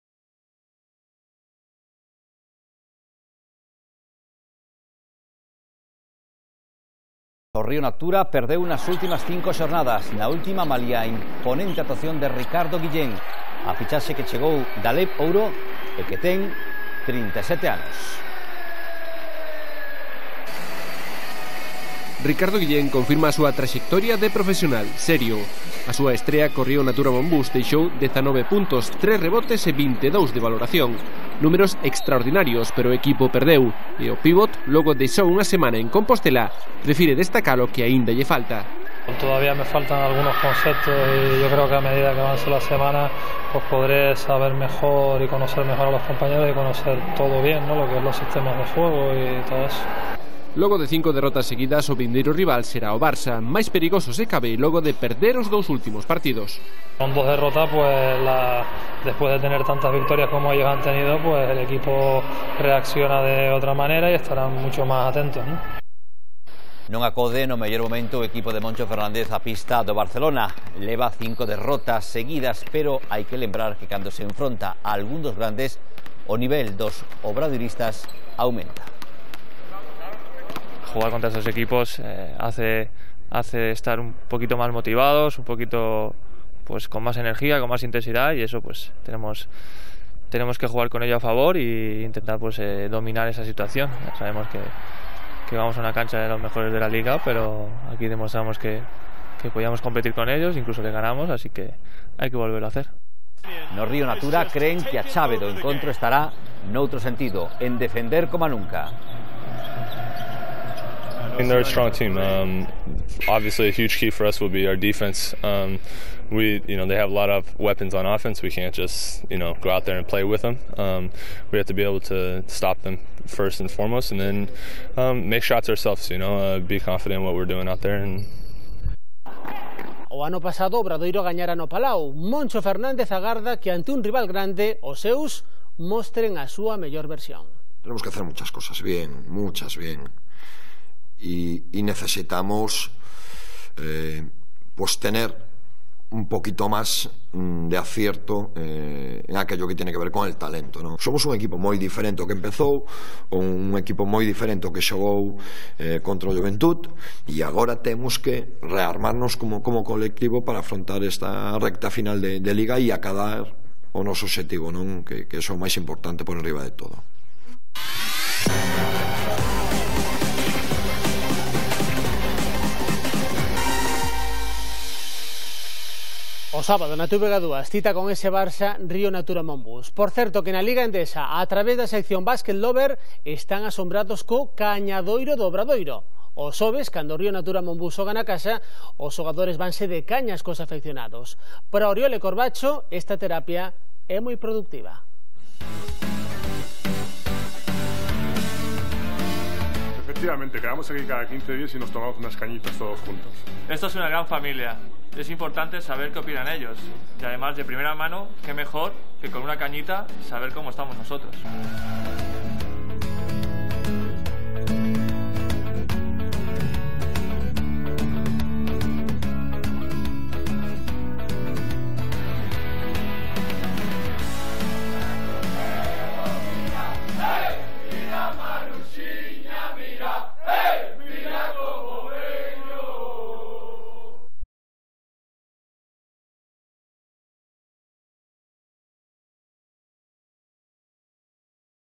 Previa J27 ACB 13/14 Vs. FC. BARCELONA
Con las declaraciones de Richi Guillén, Alberto Corbacho, Travis Peterson y Moncho Fernández.
Mención para el canhadoiro.

Créditos:
TVG